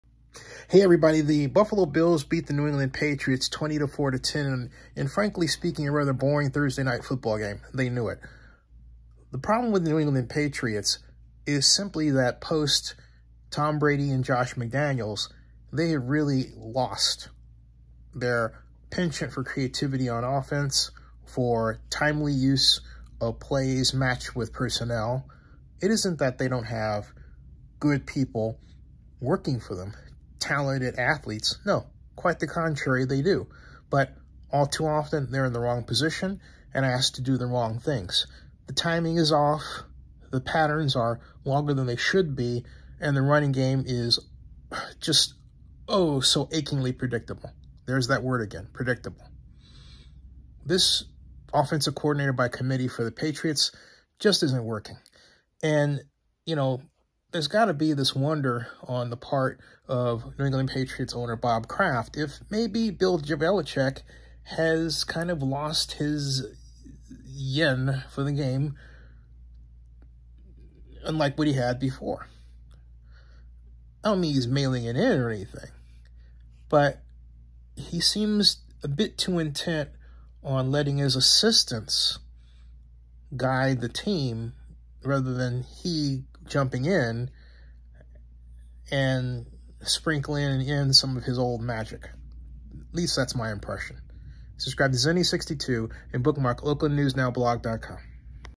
Bills Beat Patriots 24-10 As New England’s Offense Is Too Predictable On NFL TNF
Bills Beat Patriots 24-10 As New England’s Offense Is Too Predictable On NFL TNF Read our new ZennieReport.com News